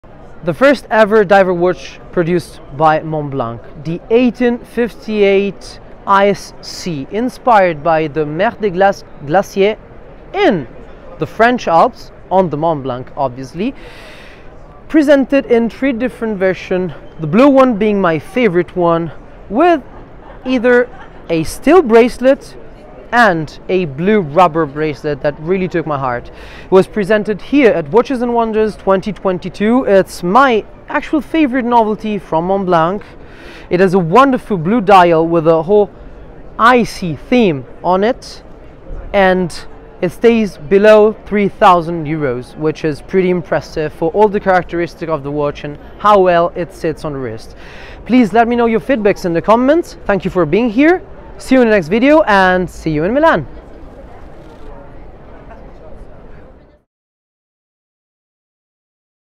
The 1858 Ice Sea from Montblanc is the watch to speak to new generation - IamCasa, Andrea Casalegno
A watch that really embodies all the aesthetics and codes of Montblanc, but with a new environment to explore: the sea.

It is the first ever diver watch conceived by Montblanc, 41mm, very modern and very young.
I’ve tried all the 3 colors with both the steel and rubber strap, but the blue on blue is literally the coolest.

Cherry on the cake: the dial finishing, that really reminds me of the Mere de Glace, a place I visited pretty often as a kid.

Wha are your thoughts about it?